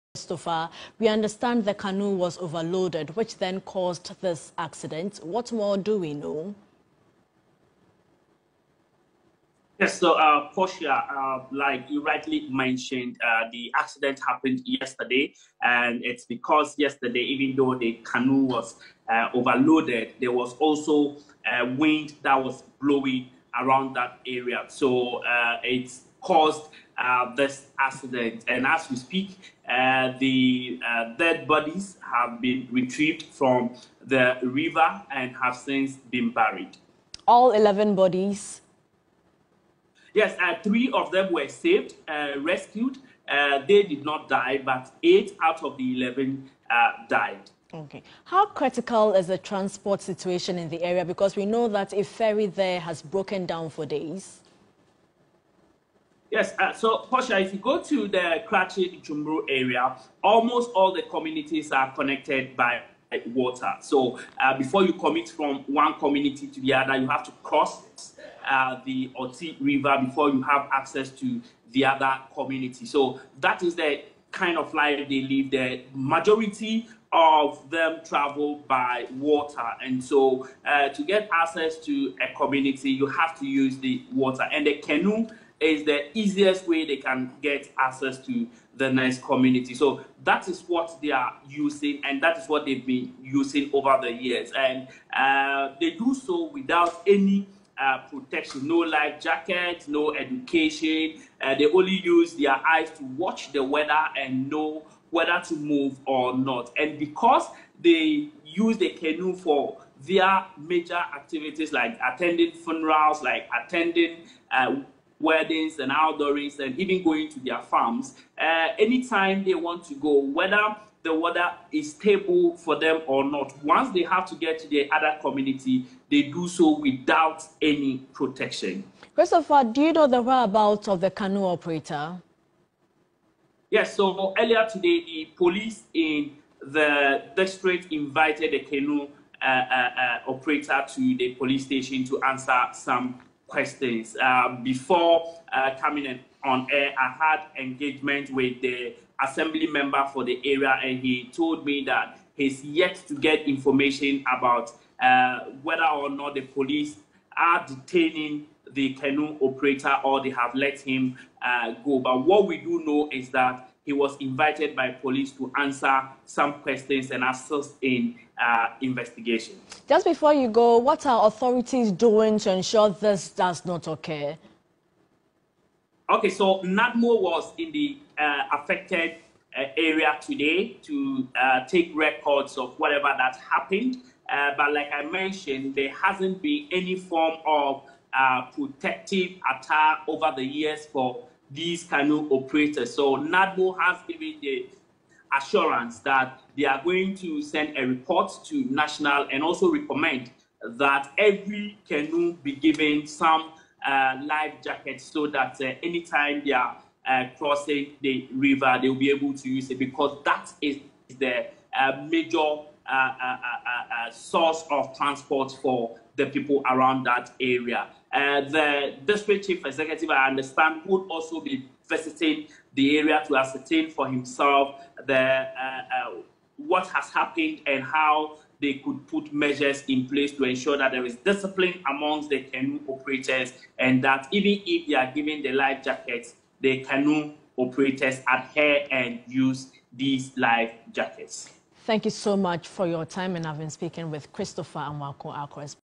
Oti canoe accident: Eight passengers confirmed dead.
8 passengers out of 11 passengers onboard a canoe have died after the canoe capsized.
Christopher Amoako reports.